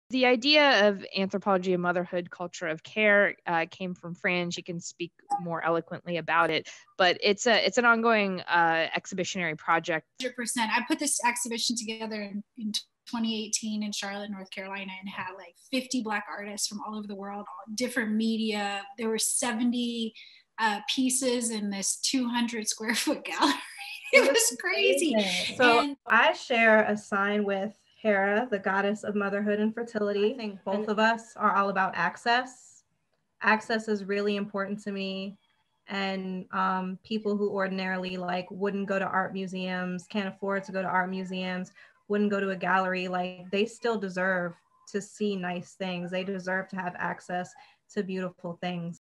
Anthropology of Motherhood Black Motherhood Panel Promo
Anthropology of Motherhood Black Motherhood Panel Promotional Video, 2020

Artists and mothers Tara Fay Coleman and Jessica Gaynelle Moss invite you to take a break from the hustle of the holidays, and join us for an afternoon of communal rest and relaxation in the gallery, as we come together as a community to celebrate and honor the challenges and joys of motherhood.

The Anthropology of Motherhood project is an ongoing curation of artwork and design that engages in the complex visual, material, emotional, corporeal and lived experiences of motherhood, care-giving, parenting, nurturing and maternal labor.

Tara Fay is an independent curator, producer, conceptual performance artist, and mama from Buffalo, NY. She serves as a board member and Associate Curator at Bunker Projects, and is a member of the Carnegie Art Associates, as well as the Associated Artists of Pittsburgh. Her curatorial process is rooted in creating space for underrepresented Black and Brown artists. She has curated exhibitions for the August Wilson African American Cultural Center, Phosphor Project Space, and most recently at Denison University in Granville, Ohio. Her forthcoming projects include an exhibition as part of the Brewhouse Association Prospectus program, and serving as a guest curator for Seton Hill University’s Harlan Gallery.

Jessica Gaynelle Moss is an artist, independent curator and arts worker. Jessica reimagines new frameworks and strategizes ways to build, maintain and sustain Black autonomous spaces devoted to the survival, resistance and healing of Black people. Her dedication to Black space stems from a housing redevelopment project that began in 2007 when she purchased a neglected property in a historic Black community and converted it into a creative community hub for Black artists and students.Jessica received a bachelors in Fine Art from Carnegie Mellon University; a masters in Arts Administration, Policy and Management from the School of the Art Institute of Chicago; and a masters in Studies of the Law from the University of Pittsburgh School of Law.